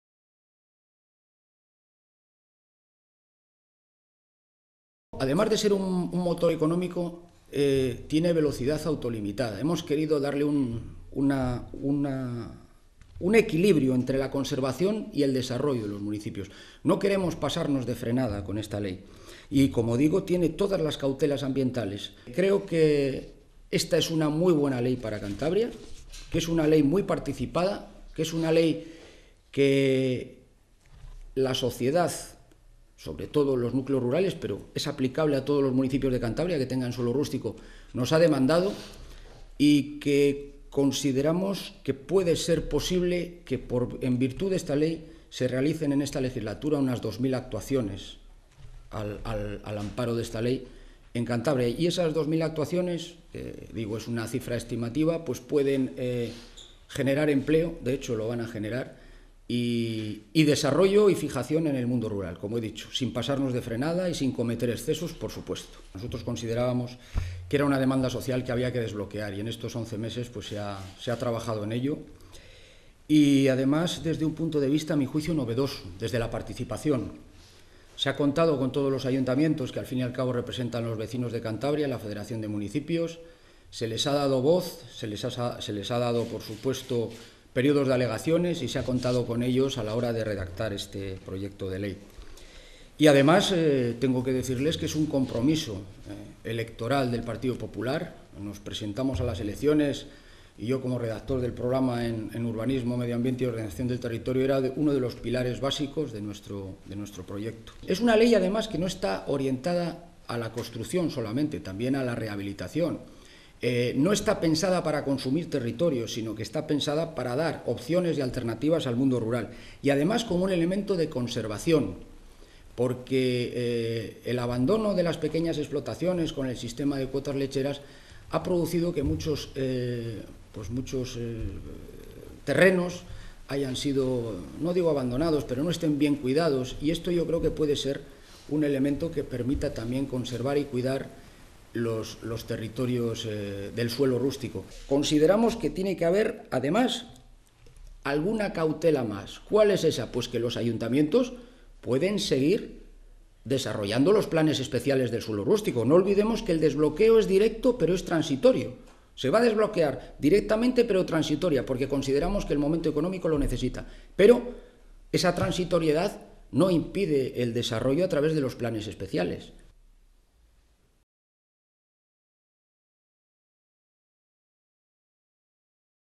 El PP dice que la Ley del Suelo es un motor de desarrollo para los municipios de la región
"Con velocidad autolimitada" para evitar excesos urbanísticos, dice el portavoz en materia de Vivienda del Partido Popular, José Manuel Igual.